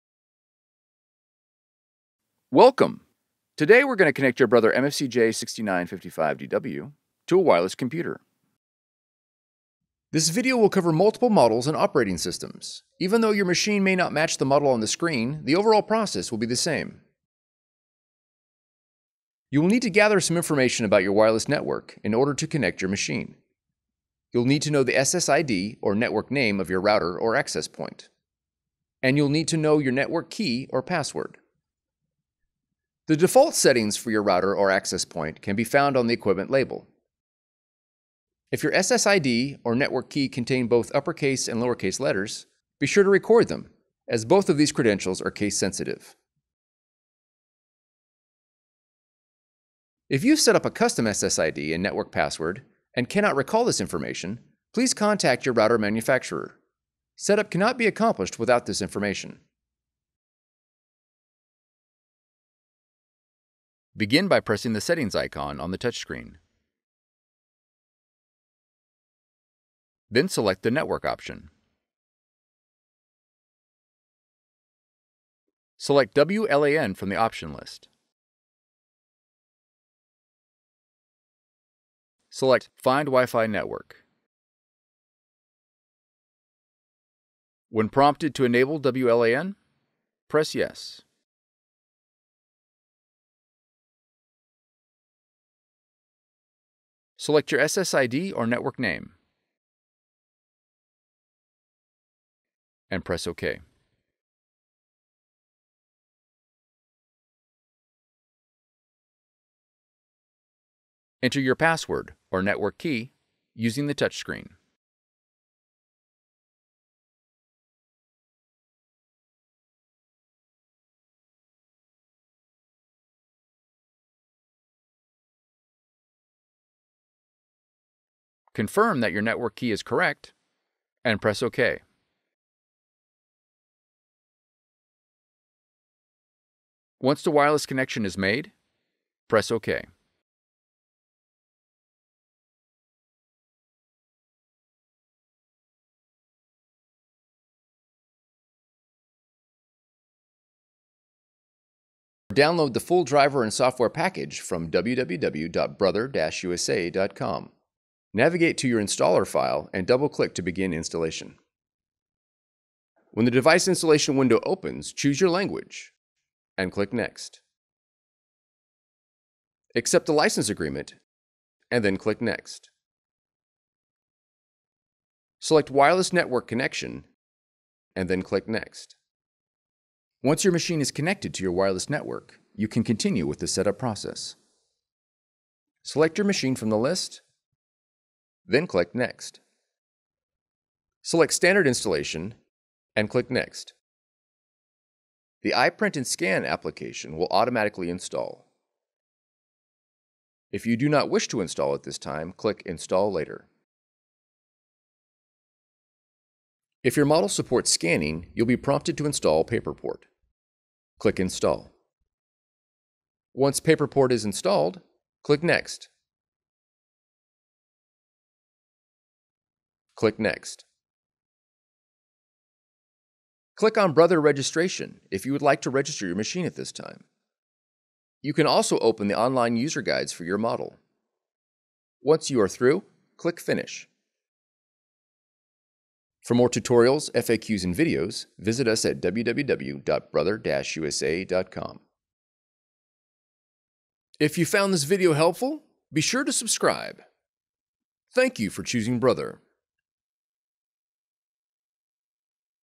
MFCJ6955DW wireless setup from control panel - Windows
Learn how to set up your Brother MFC-J6955DW inkjet printer on a wireless network in this video tutorial. This method will use your machine’s control panel to establish the wireless connection.

Models covered:
Brother MFCJ6955DW

Chapters:
Intro 00:00
Models covered 00:10
Find network name and password 00:20
Select network name 01:30
Enter network password 01:45
Download the software 02:30
Running the Installer software 02:40
Install iPrint&Scan 3:15
PaperPort Install 03:25
User’s Guide and Registration 03:40

Transcript:
Welcome, today we’re going to earn how to set up your Brother MFC-J6955DW to a wireless network using the machine control panel

Begin by pressing the settings icon on the touchscreen.

Then select the Network option

Select “WLAN” from the option list (Enable WLAN screen)

Select “Find Wi-Fi Network”

Enter your Network Key, using the touchscreen.

Download the full driver and software package Navigate to your installer file and double-click to begin installation.

Once your machine is connected to your wireless network you can continue with the setup process

Select "Wireless network connection (Wi-Fi)" and click "Next"

The iPint&Scan application will automatically install. If you do not wish to install at this time click “Install Later”

If your model supports scanning you will be prompted to install PaperPort.

Click "Install."

Click "Next"

Click on Brother registration to register your machine at this time. You can also open the online user guides for your model
Once you are through, click "Finish."

Customers also searched:
Brother MFC J6955DW wireless control panel Windows
Brother MFC J6955DW wireless setup control panel Windows